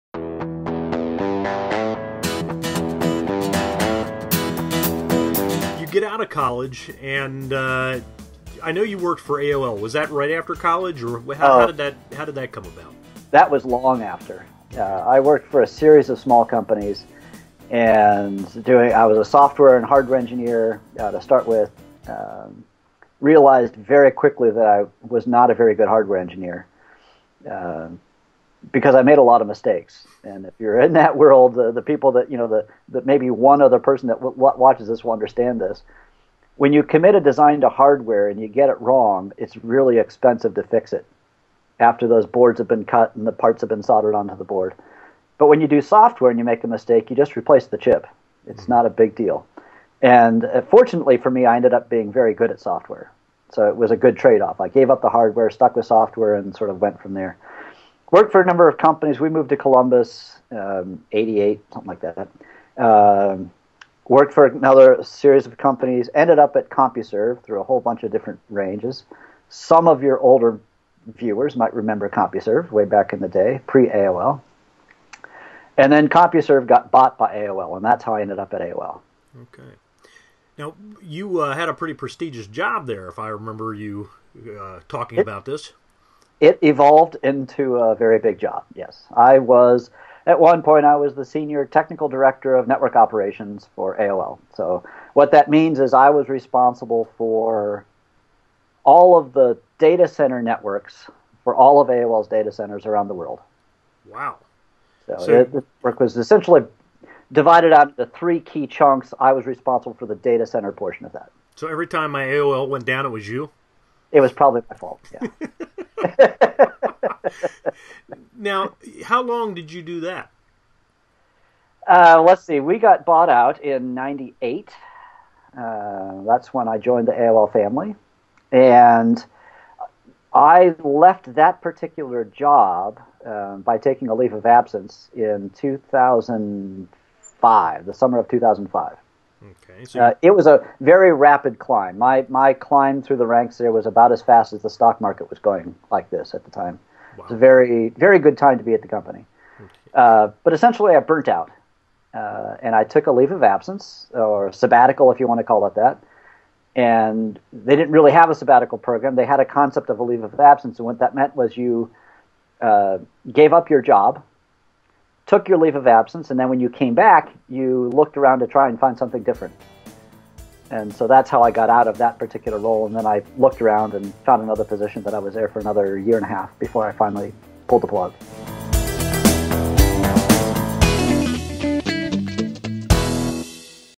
Ventriloquist David Crone Interview Part 4
Ventriloquist David Crone is interviewed for the Learn Ventriloquism Blog.  Part 4 of 15